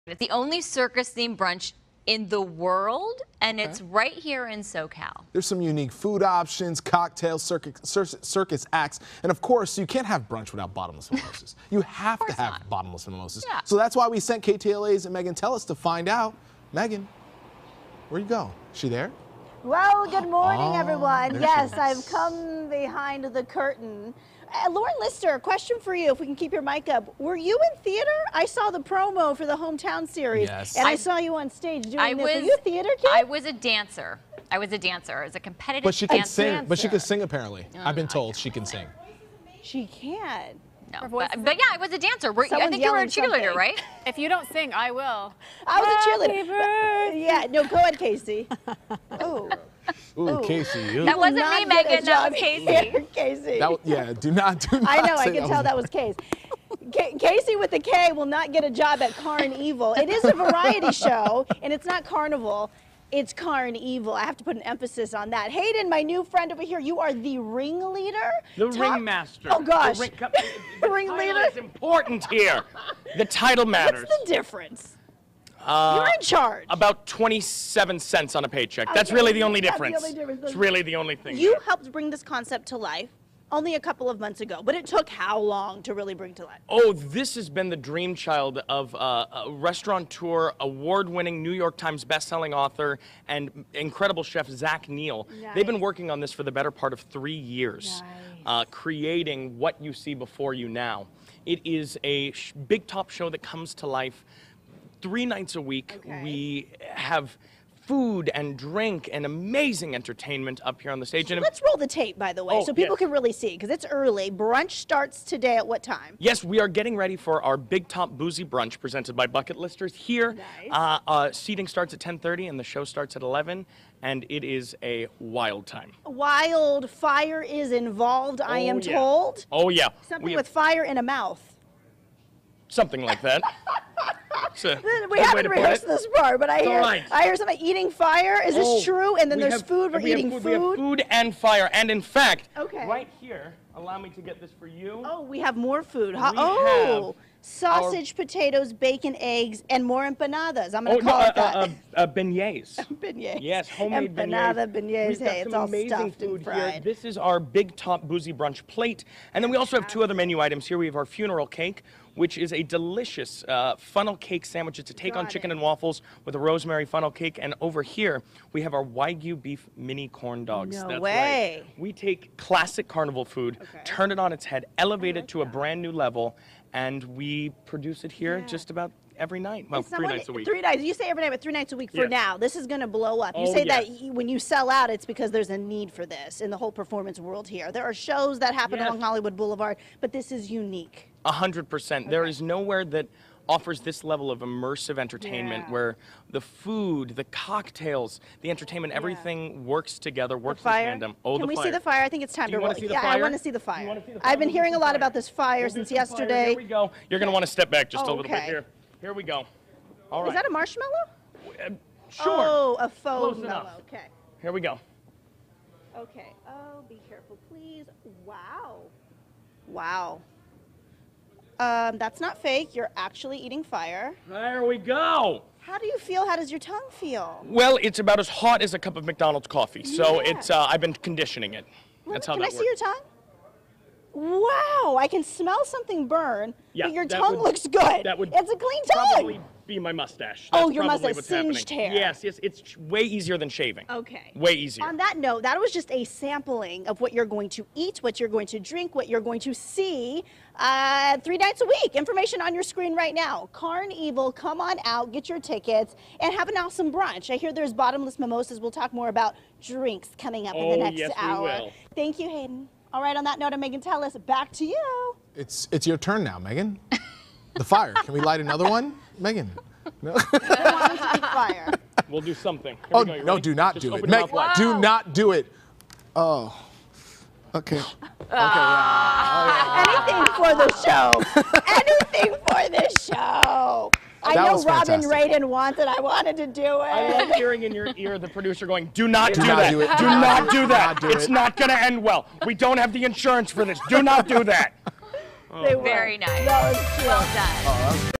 Karnevil brings Cirque Brunch to Los Angeles
Megan Telles reports for the KTLA 5 Weekend Morning News on Aug. 7, 2022.